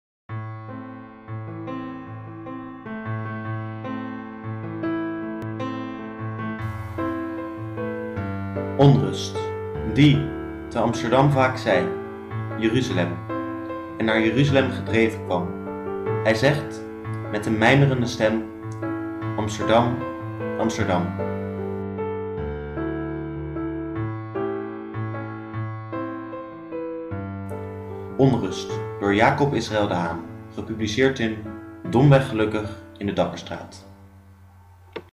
Onrust - Jacob Israël de Haan
Het gedicht onrust van Jacob Israël de Haan met passende foto's en muziek gemaakt voor een poëziedossier.